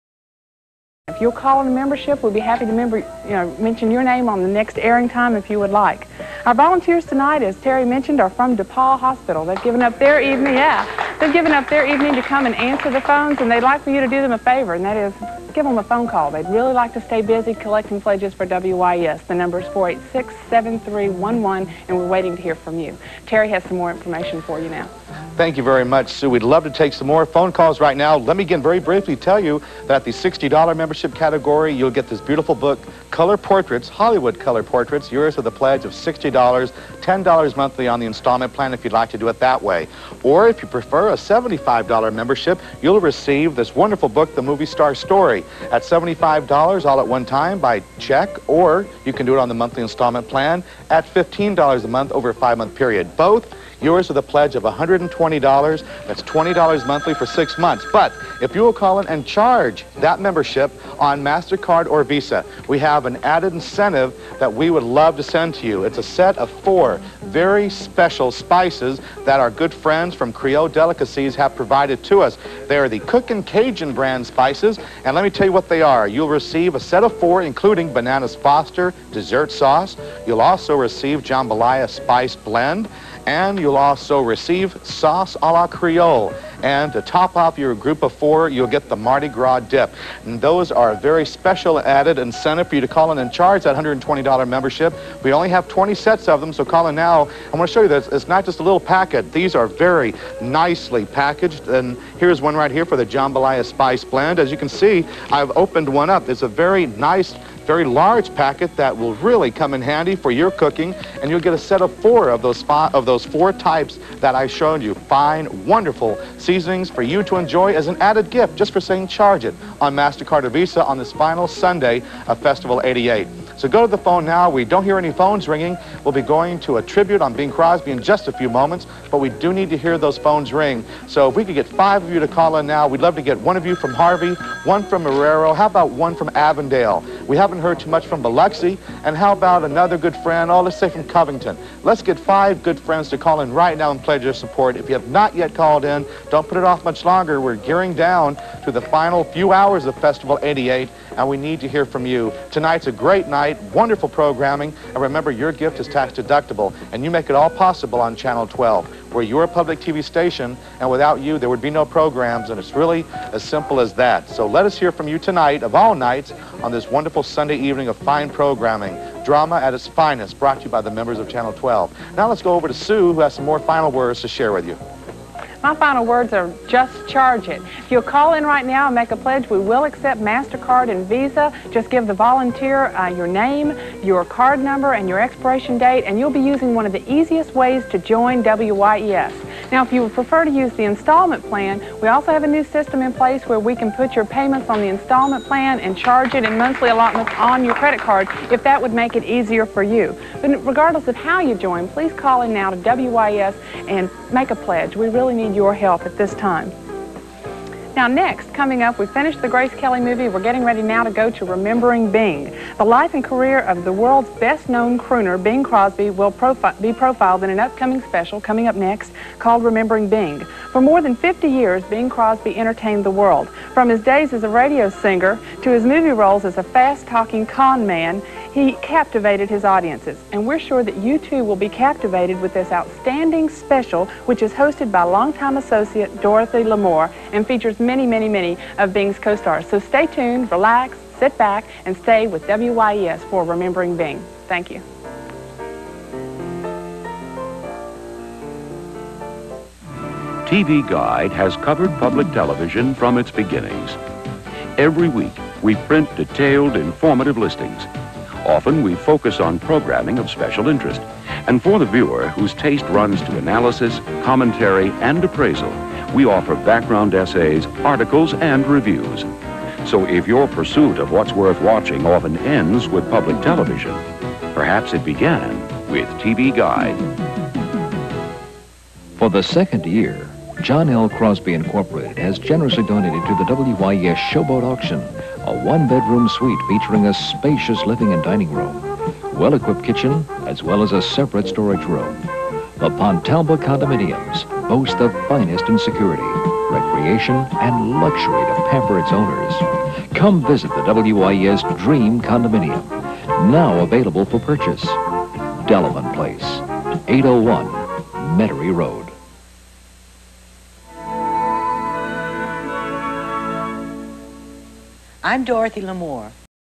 3/27/1988 WYES Channel 12 New Orleans Pledge Drive
I this pledge drive took place on March 27th 1988.  (Thank you Chris Hadley for that date) The clip aired before  "Remembering Bing Crosby" on WYES. WYES is the PBS station in New Orleans.